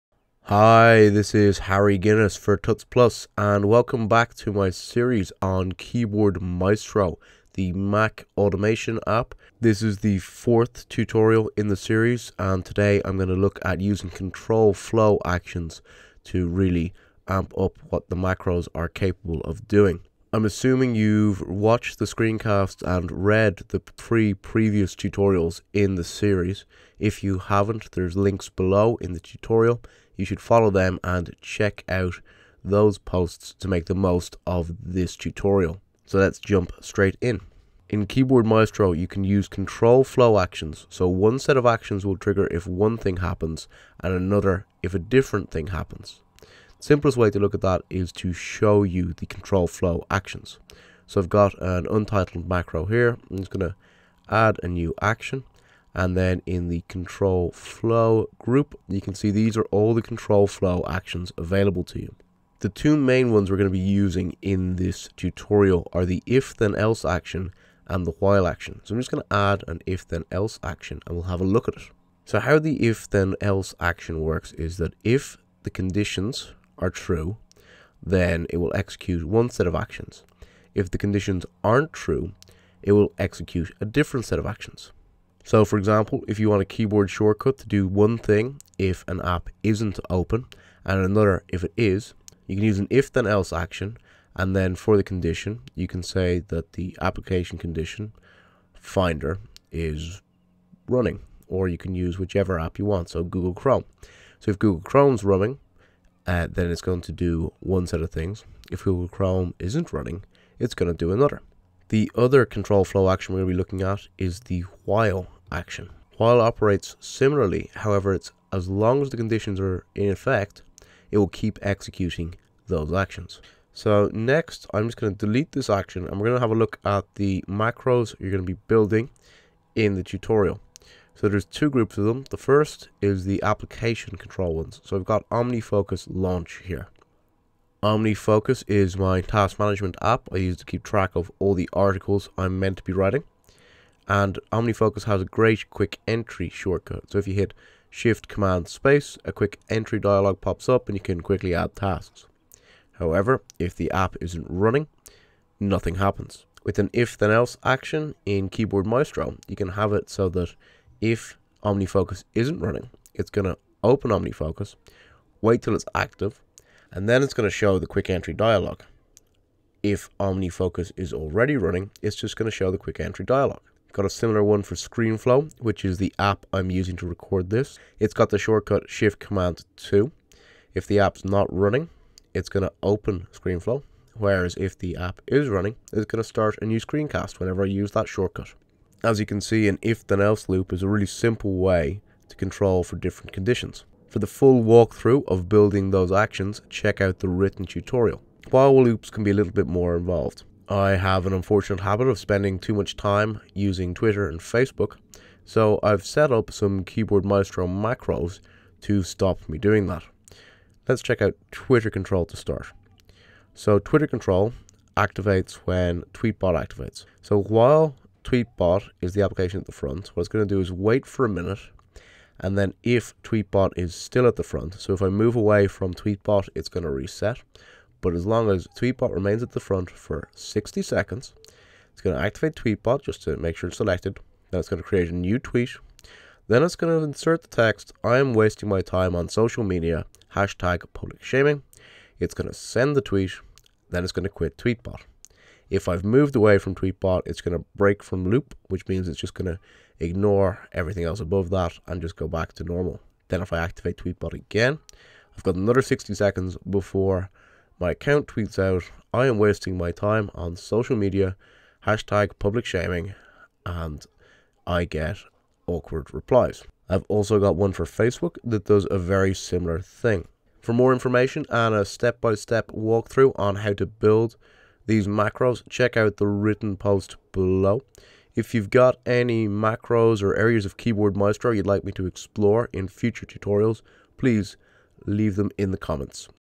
Keyboard Maestro IV - Control Flow
Keyboard Maestro is one of the most powerful and versatile Mac apps available. By creating strings of actions—or macros—that connect different apps you can get almost total control over OS X. In the previous three tutorials I introduced Keyboard Maestro, looked at how to launch apps intelligently and, most recently, looked at how to use situational triggers.

If you've been following along with the previous tutorials you should have a good feel for how to create simple macros with Keyboard Maestro. In this tutorial I'll look at creating much more involved macros using Control Flow actions, one of Keyboard Maestro's most powerful features.